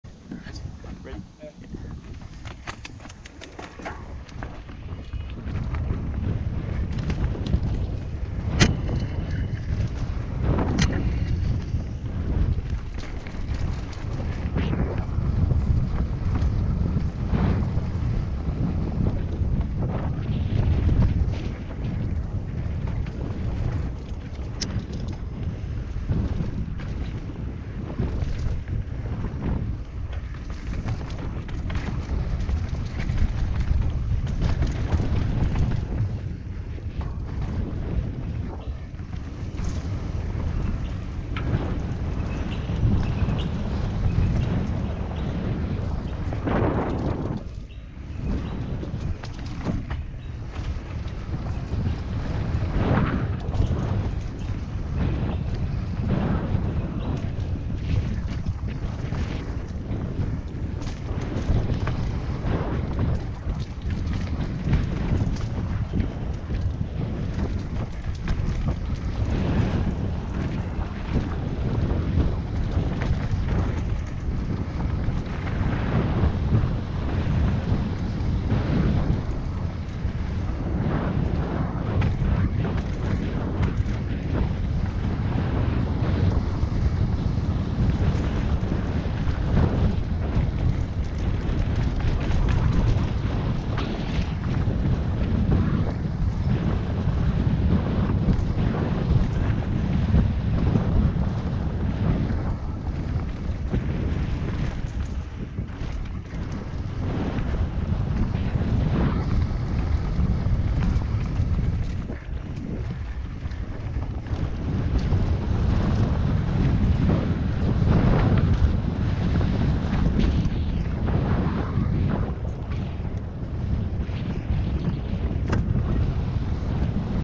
MTB Majura Pines Rock Lobster: Stabilized
Rock Lobster trail at Majura Pines MTB Park. Filmed with a stabilized GoPro.